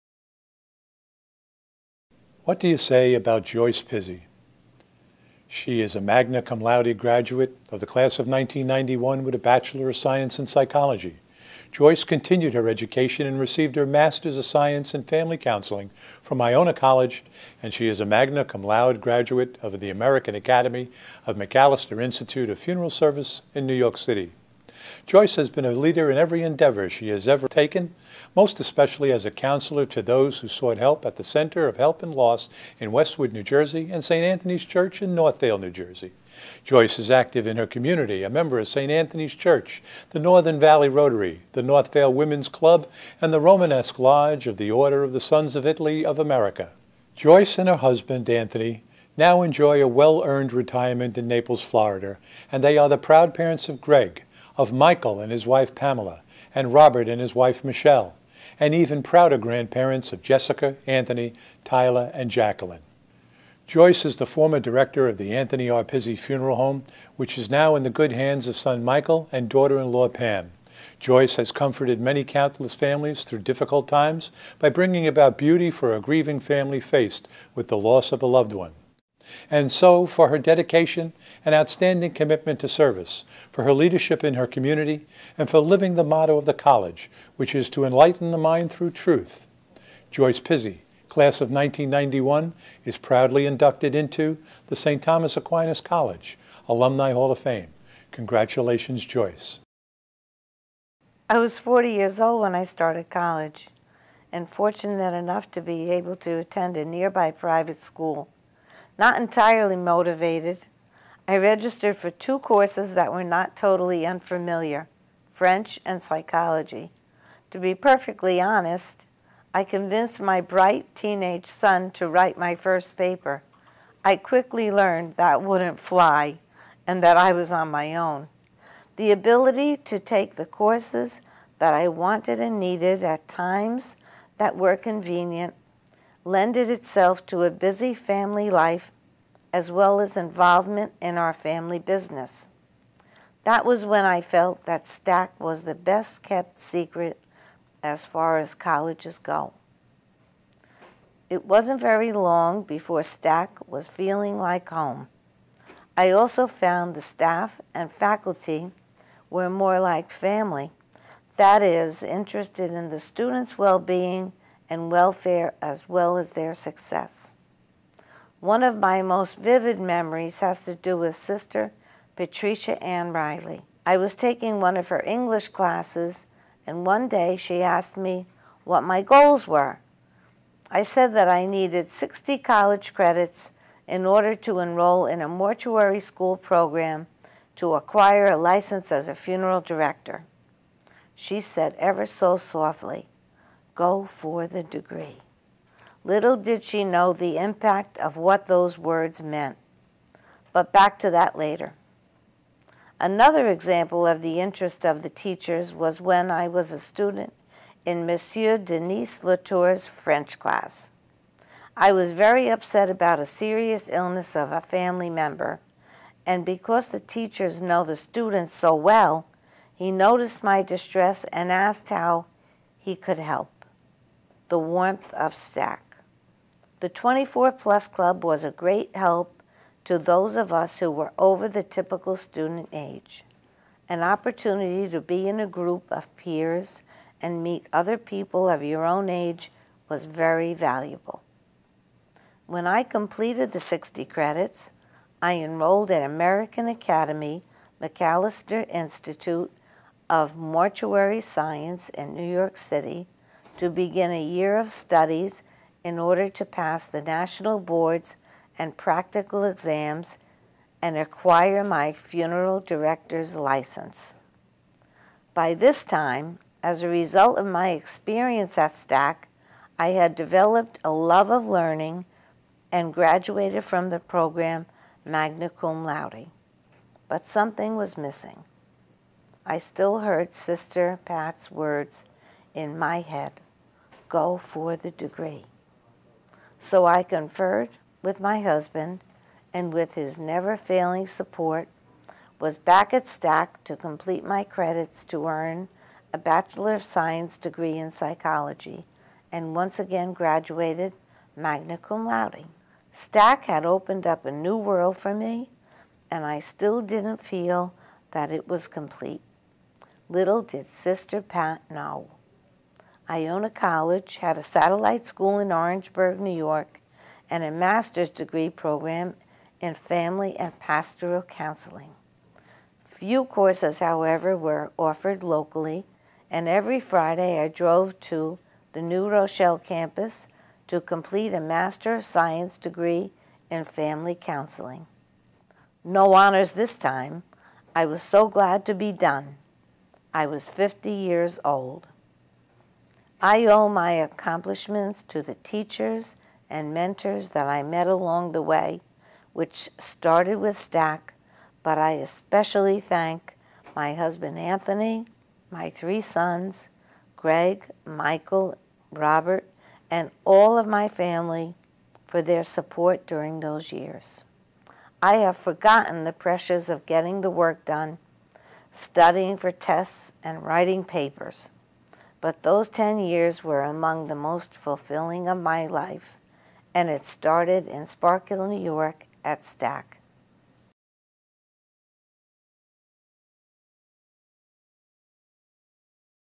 2016 STAC Hall of Fame, Joyce Pizzi
Joyce Pizzi, Class of 1991, inducted by Mr. Kevin Duignan, STAC VP for Institutional Advancement.

Joyce is a funeral director and former owner of Anthony R. Pizzi Funeral Home in Northvale, NJ.